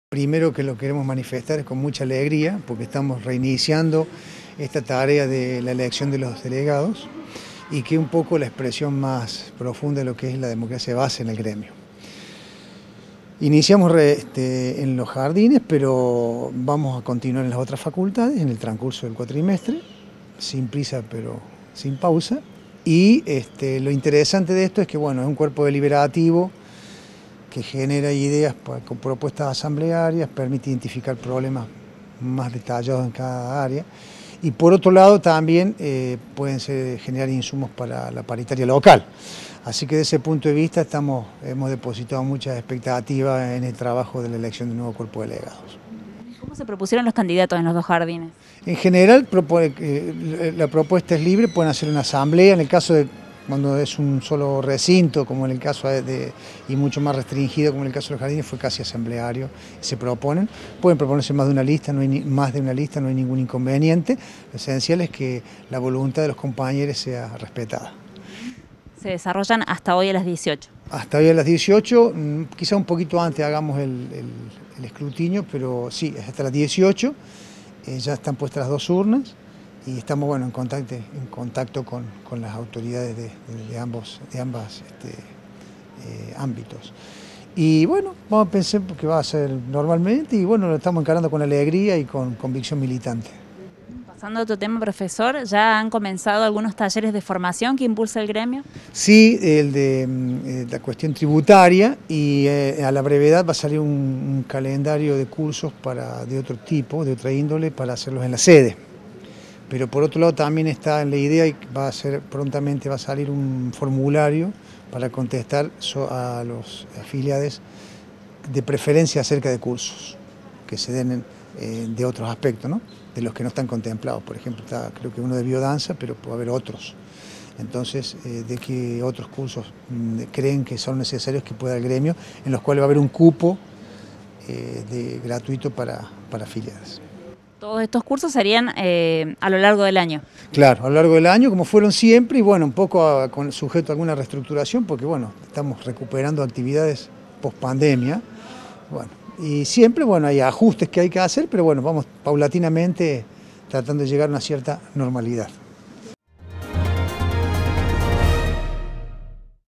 ALEJANDRO LARRIESTRA – SECRETARIO GREMIAL
ELECCIÓN DE DELEGADOS GREMIALES EN EL JARDIN MATERNAL “RAYITO DE SOL” Y JARDÍN INICIAL “ROSARIO VERA PEÑALOZA”